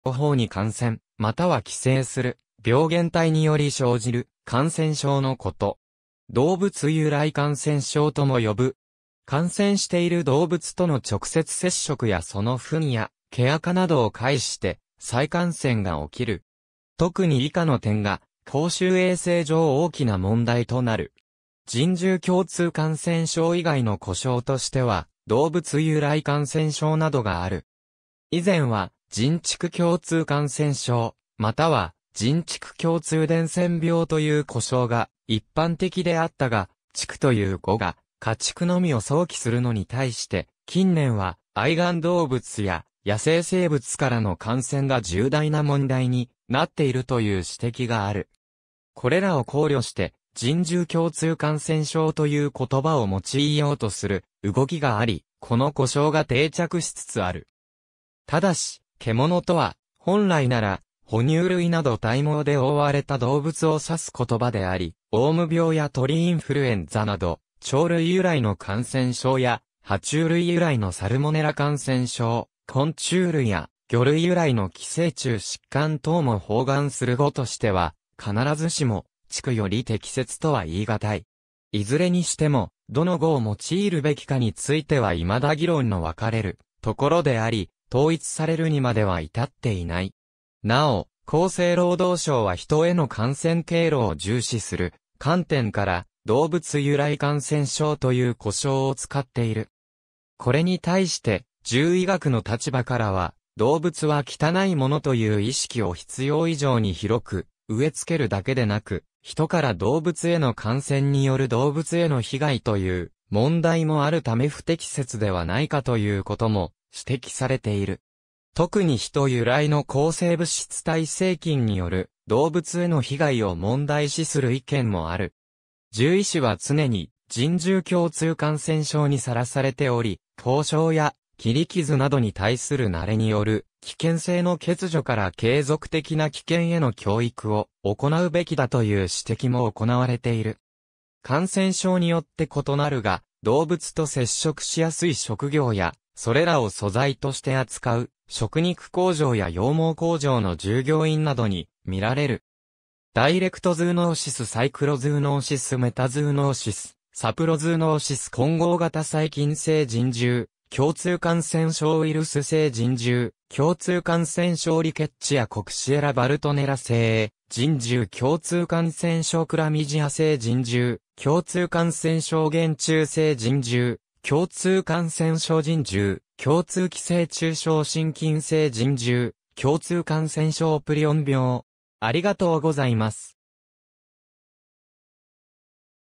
人獣共通感染症
グローバル・ヘルス
人間と野生動物、家畜に共通して感染する感染症 人獣共通感染症（じんじゅうきょうつうかんせんしょう：ズーノーシス（zoonosis））は、ヒトとそれ以外の脊椎動物の両方に感染または寄生する病原体により生じる感染症のこと。
動物由来感染症とも呼ぶ（呼称についてを参照）。
感染している動物との直接接触やその糞や毛垢などを介して再感染が起きる。
特に以下の点が公衆衛生上大きな問題となる。
人獣共通感染症以外の呼称としては動物由来感染症などがある。
以前は人畜共通感染症または人畜共通伝染病という呼称が一般的であったが、「畜」という語が家畜のみを想起するのに対して、近年は愛玩動物（ペット）や野生生物からの感染が重大な問題になっているという指摘がある。
これらを考慮して、人獣共通感染症という言葉を用いようとする動きがあり、この呼称が定着しつつある。
ただし、「獣」とは本来なら哺乳類など体毛で被われた動物を指す言葉であり、オウム病や鳥インフルエンザなど鳥類由来の感染症や、爬虫類由来のサルモネラ感染症、昆虫類や魚類由来の寄生虫疾患等も包含する語としては必ずしも「畜」より適切とは言い難い。
いずれにしても、どの語を用いるべきかについては未だ議論の分かれるところであり、統一されるにまでは至っていない。
なお、厚生労働省はヒトへの感染経路を重視する観点から動物由来感染症という呼称を使っている。
これに対して獣医学の立場からは、「動物は汚いもの」という意識を必要以上に広く植え付けるだけでなく、ヒトから動物への感染（ヒト由来感染症）による動物への被害という問題もあるため不適切ではないかということも指摘されている。
特にヒト由来の抗生物質耐性菌による動物への被害を問題視する意見もある。
獣医師は常に人獣共通感染症にさらされており、咬傷や切り傷などに対する慣れによる危険性の欠如から継続的な危険への教育を行うべきだという指摘も行われている。
感染症によって異なるが、動物と接触しやすい職業や、それらを素材として扱う食肉工場や羊毛工場の従業員などに見られる。
ダイレクトズーノーシス（direct zoonosis） サイクロズーノーシス（cyclo-zoonosis） メタズーノーシス（meta-zoonosis） サプロズーノーシス（sapro-zoonosis） 混合型 細菌性人獣共通感染症 ウイルス性人獣共通感染症 リケッチア・コクシエラ・バルトネラ性人獣共通感染症 クラミジア性人獣共通感染症 原虫性人獣共通感染症 人獣共通寄生虫症 真菌性人獣共通感染症 プリオン病